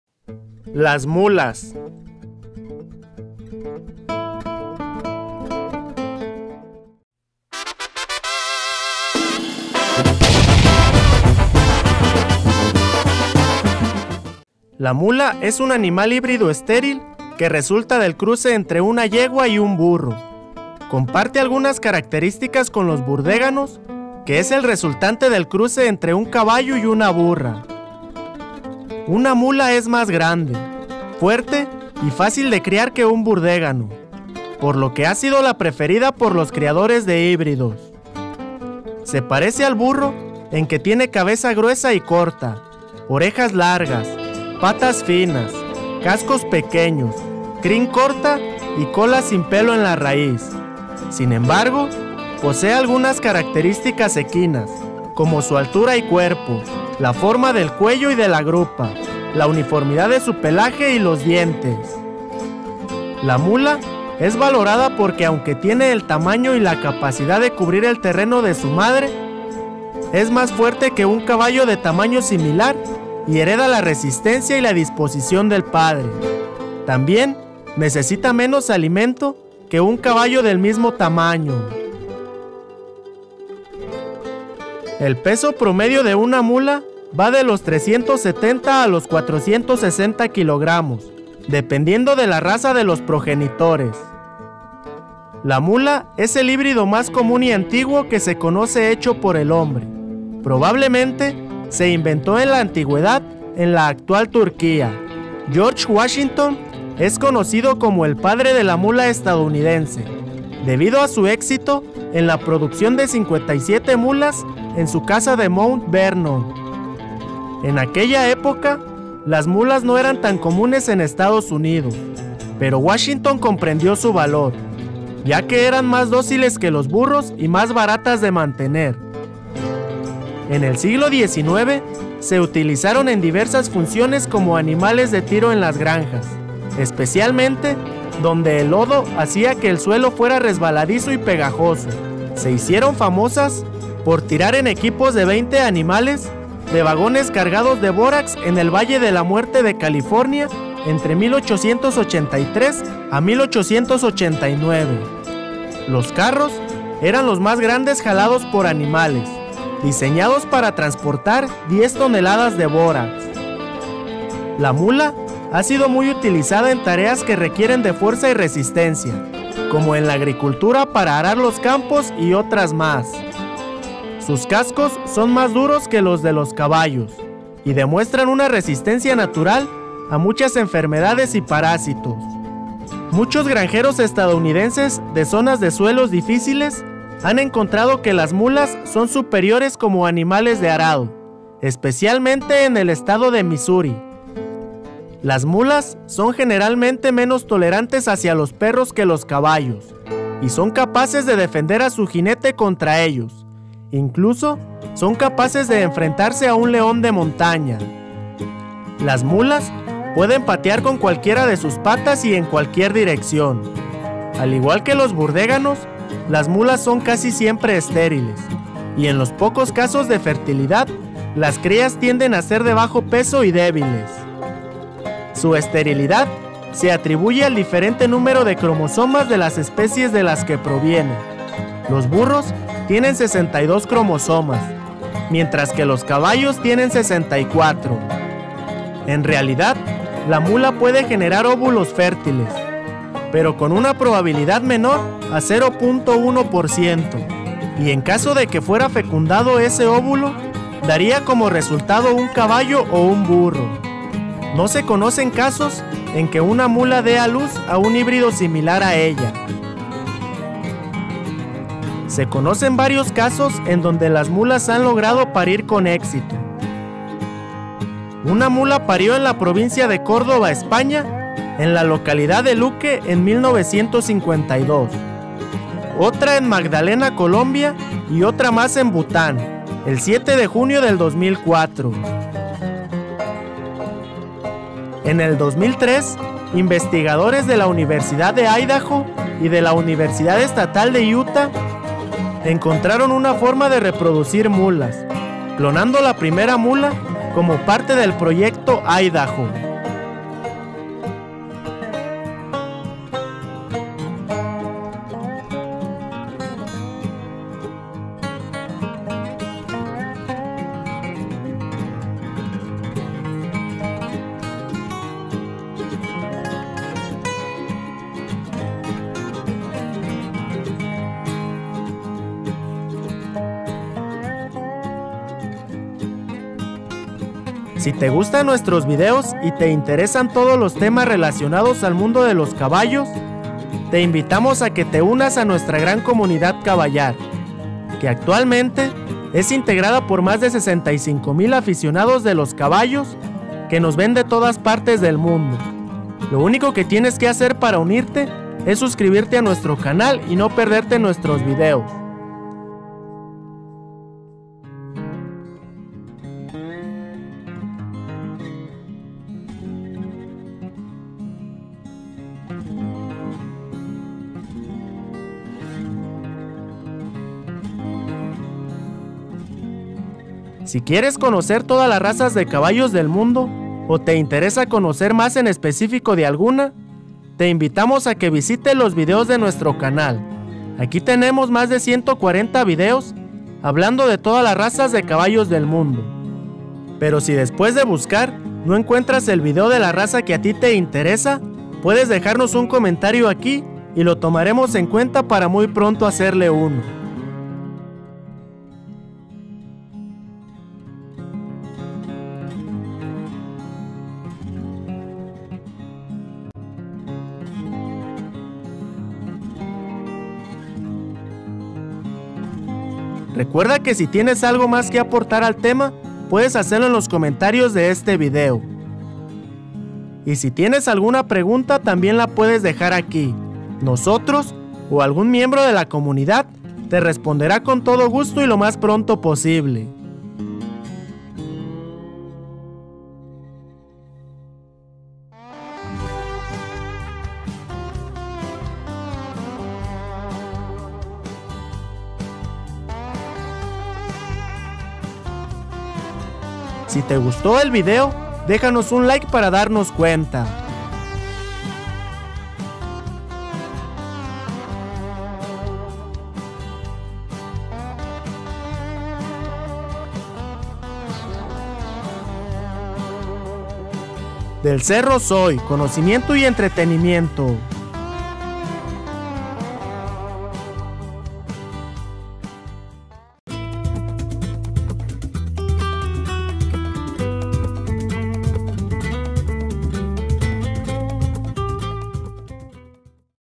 Las Mulas 🐴-Del Cerro Soy
Del Cerro Soy:
Las mulas tienen la fama de ser infértiles, pero en realidad, han existido casos de mulas que han logrado parir, en este video te lo mostramos